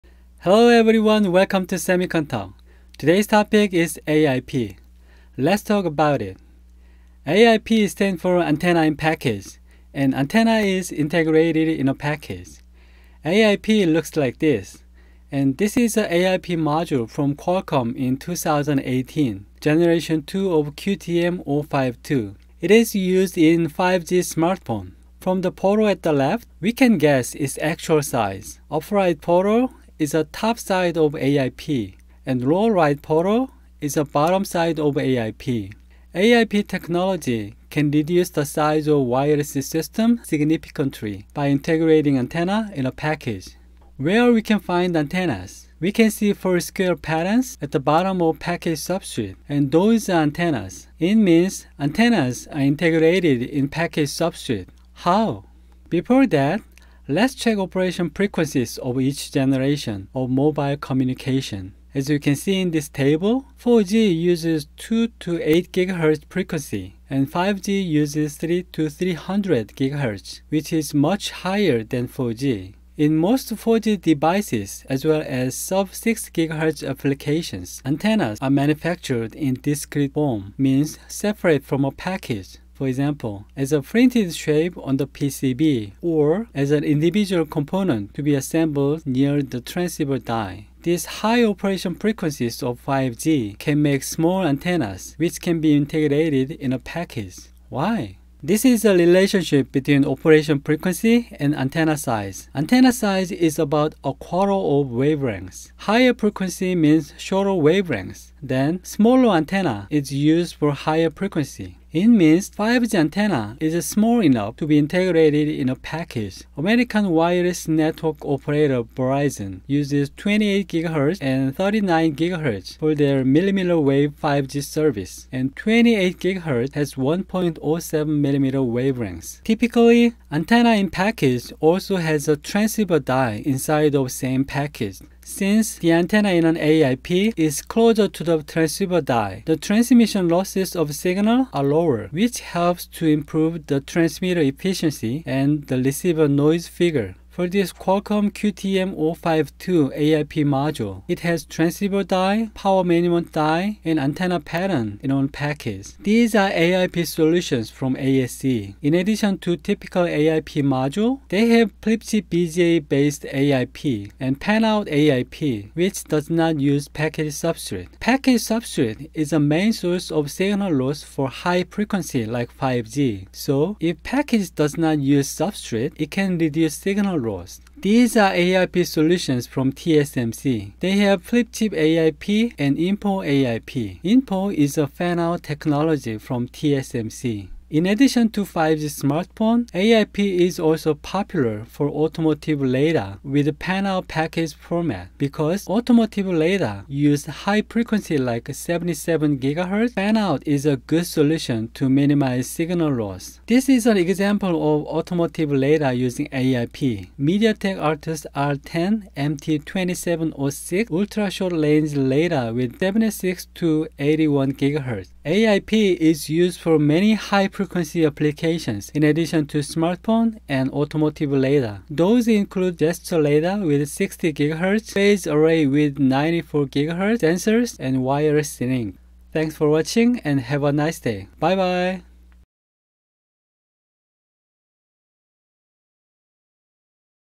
[Eng Sub] AiP (Antenna in Package)
[Typo correction]
2:58 - The left picture is InFO AiP and the right picture is FC AiP
1. AiP (Antenna in Package)
   : Qualcomm QTM052 Gen. 2
   : Antenna Pattern in AiP
   : Frequency vs. Antenna Size
2. Configurations
   : AiP = Patch Antennas + Transceiver + PMIC
   : ASE
   : TSMC
3. Applications
   : 5G Smartphone, Automotive Radar, Gesture Radar, Phased arrays, Sensors, Wireless links